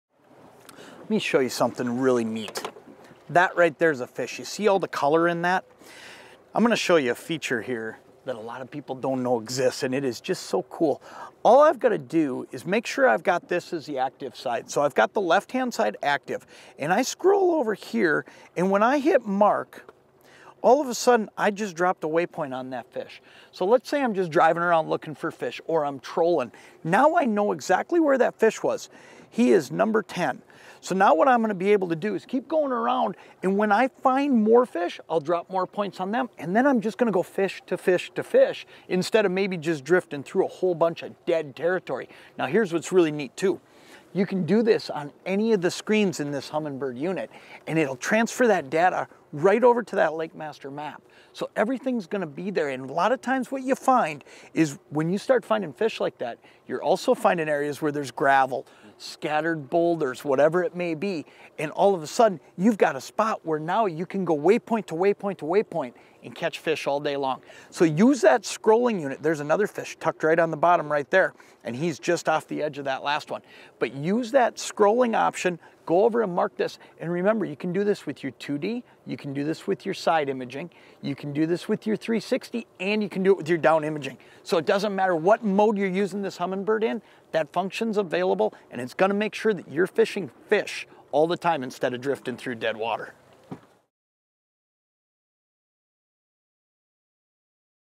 "How to Use the Scrolling Function for Marking Waypoints on Humminbird Lakemaster Sonar and GPS Unit
Jon Thelen demonstrates the process for using the scrolling and marking waypoint function on a Humminbird Lakemaster sonar and GPS unit.